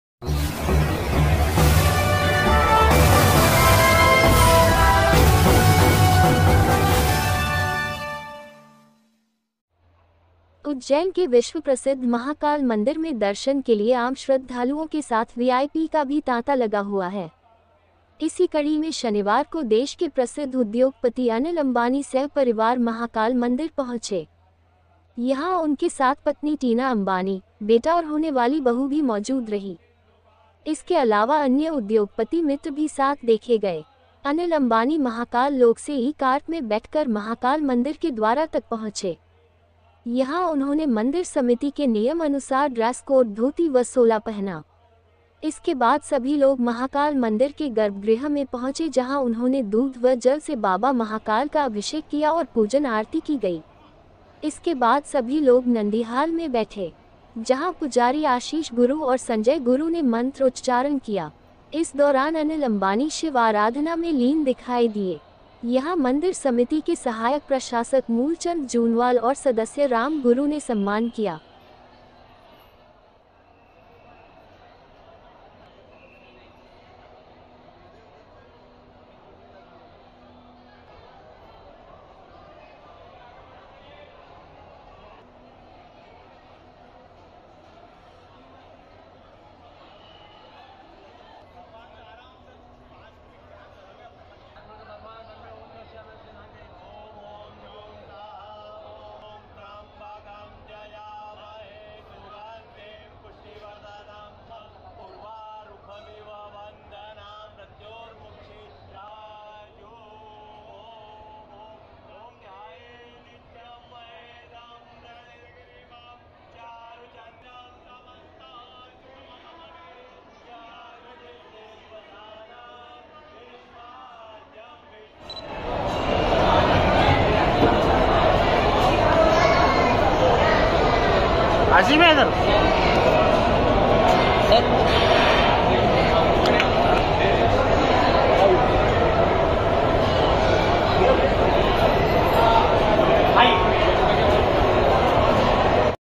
UJJAIN प्रसिद्ध उद्योगपति अनिल अंबान,पत्नी व बेटे के साथ पहुंचे महाकाल मंदिर,
Khabar Ujjain
प्रसिद्ध उद्योगपति अनिल अंबान,  पत्नी व बेटे के साथ पहुंचे महाकाल मंदिर,
गर्भगृह में पहुंचकर किया पूजन अभिषेक,